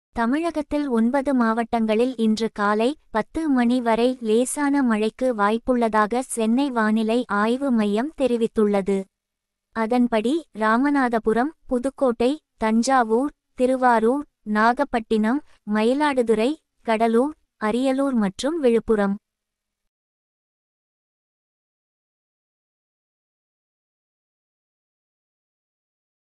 breakingnews தமிழகத்தில் 9 மாவட்டங்களில் காலை 10 மணி வரை லேசான மழைக்கு வாய்ப்பு | Tamil News
தமிழகத்தில் 9 மாவட்டங்களில் காலை 10 மணி வரை லேசான மழைக்கு வாய்ப்பு
எடப்பாடி,எடப்பாடிபழனிச்சாமி,ஓபன்னீர்செல்வம்,சசிகலா,ஸ்டாலின்,அதிமுக,திமுக,பாஜக, இன்றைய  செய்திகள், சத்தியம் செய்திகள், தலைப்புச் செய்திகள்.